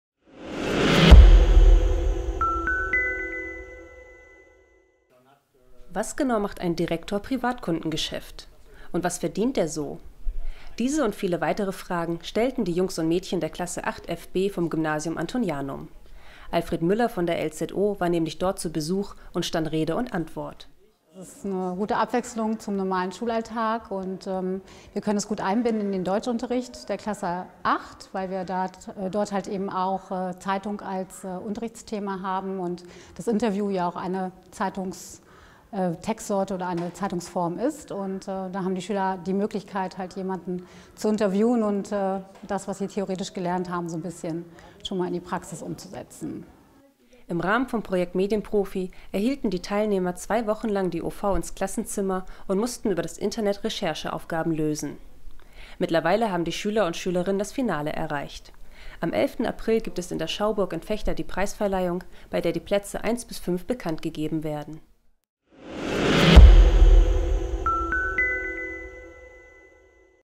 Besuch im Gymnasium Antonianum
Im Rahmen vom Projekt Medienprofi gab es im Gymnasium Antonianum Besuch: Alfred Müller von der LzO stand den Schülern und Schülerinnen Rede und Antwort in einem Interview. Weitere Infos zum Thema gibt es in der OV am Freitag, den 18.03. und im epaper.